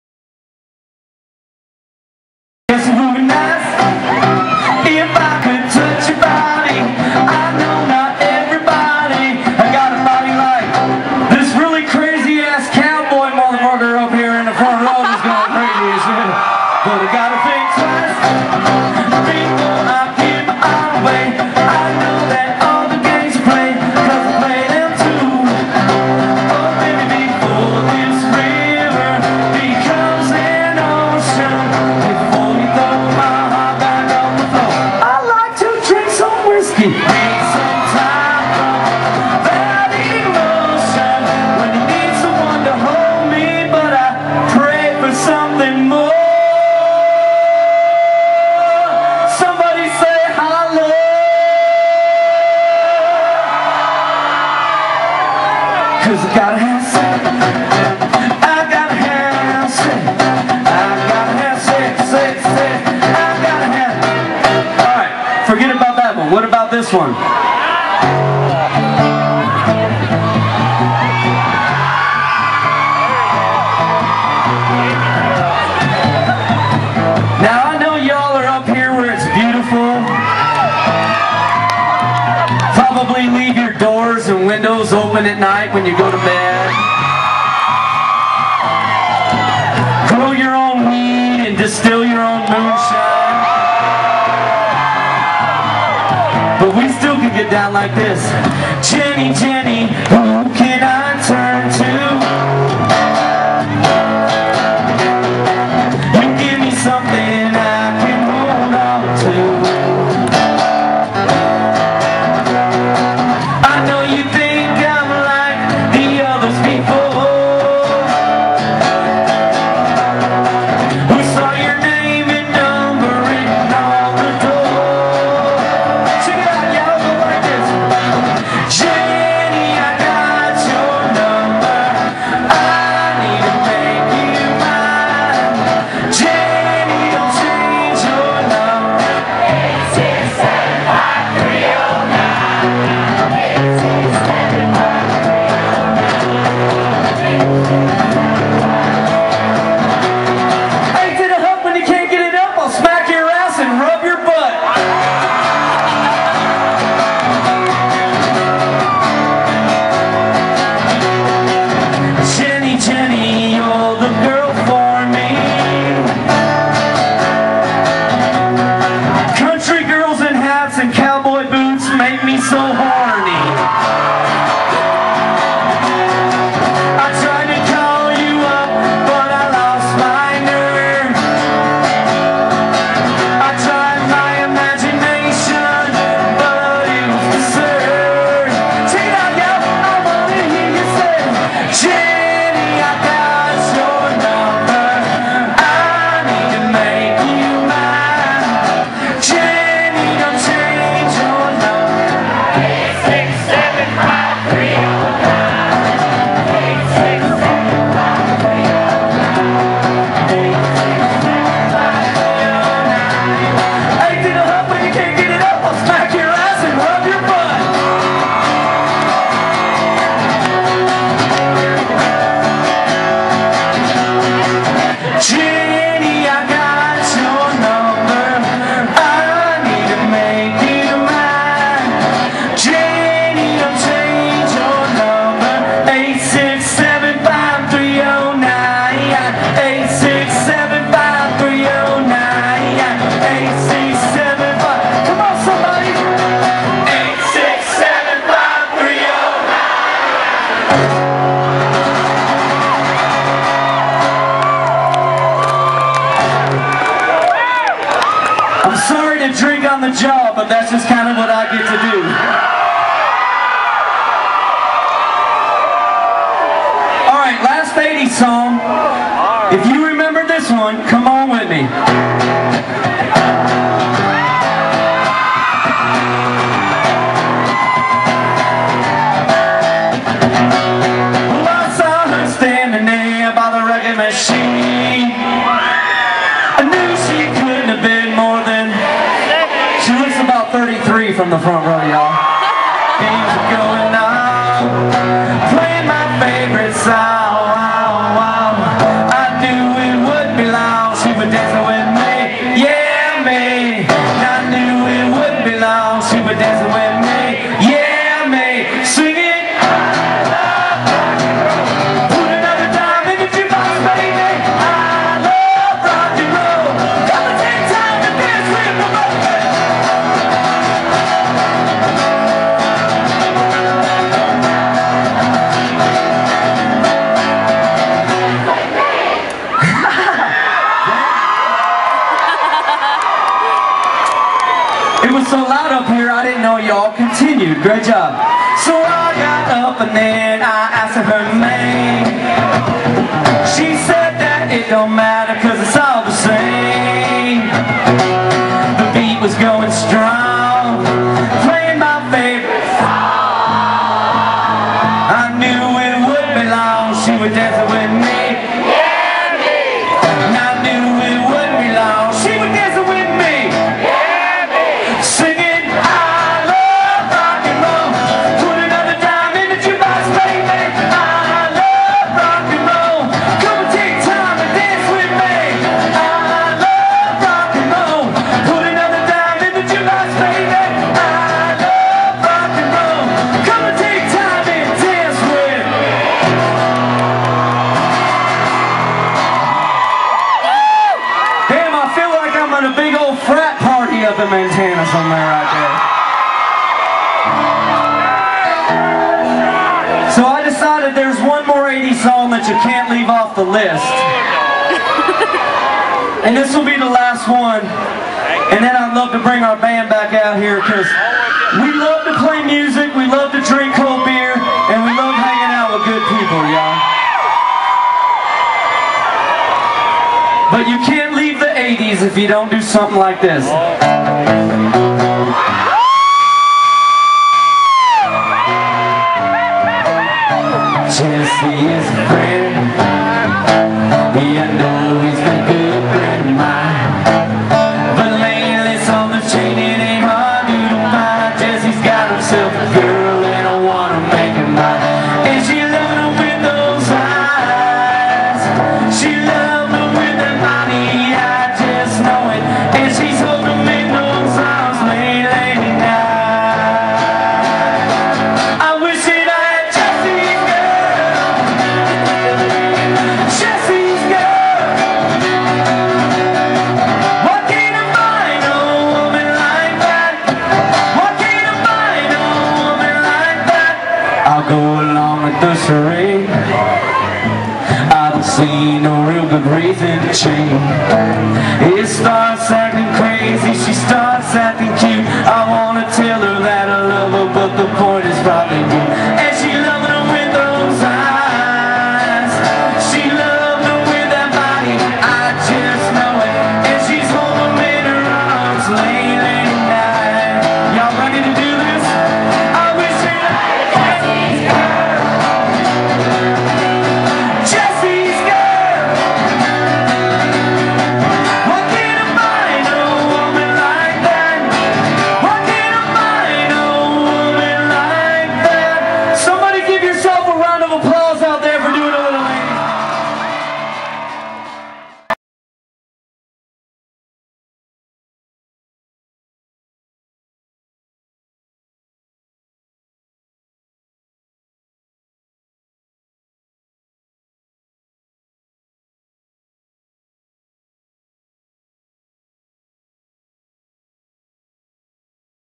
Trailer Choir Butter's 80's Tunes
Butter from the Trailer Choir singing some 80's tunes at the Headwaters Country Jam 2011 in Three Forks Montana